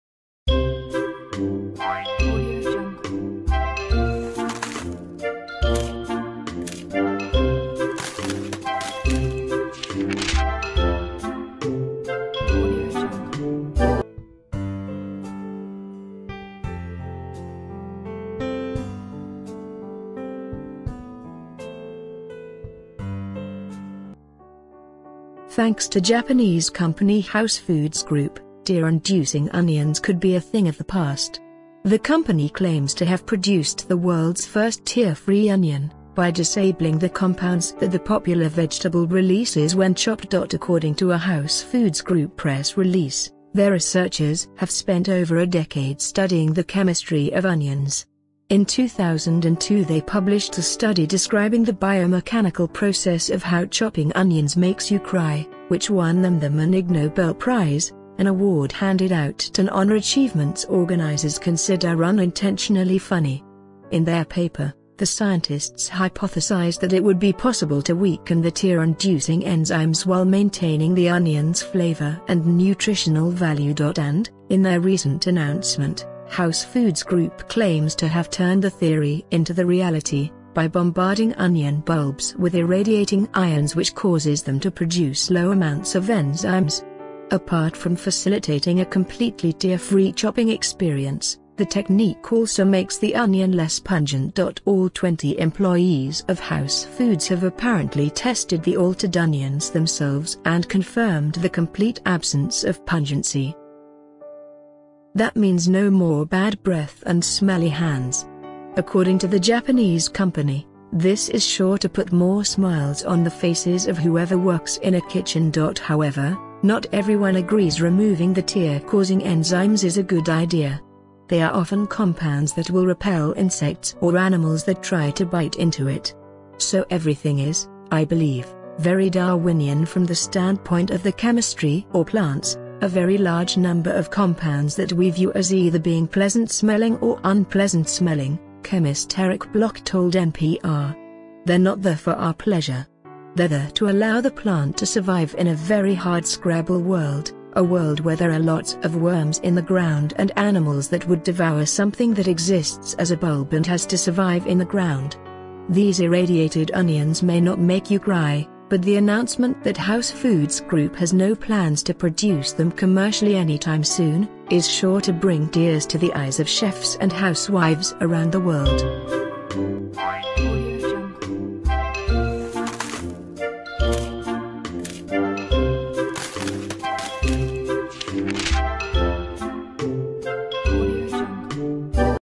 Finally! Japanese Company Creates Onion That Doesn’t Make You Cry
Thanks to Japanese company House Foods Group, tear-inducing onions could be a thing of the past! The company claims to have produced the world’s first ‘tear-free’ onion, by disabling the compounds that the popular vegetable releases when chopped.

According to a House Foods Group press release, their researchers have spent over a decade studying the chemistry of onions. In 2002 they published a study describing the biomechanical process of how chopping onions makes you cry, which won them them an Ig Nobel Prize – an award handed out to honor achievements organizers consider unintentionally funny.  In their paper, the scientists hypothesized that it would be possible to weaken the tear-inducing enzymes while maintaining the onion’s flavor and nutritional value.

And, in their recent announcement, House Foods Group claims to have turned the theory into a reality, by bombarding onion bulbs with irradiating ions which causes them to produce low amounts of enzymes. Apart from facilitating a completely tear-free chopping experience, the technique also makes the onion less pungent.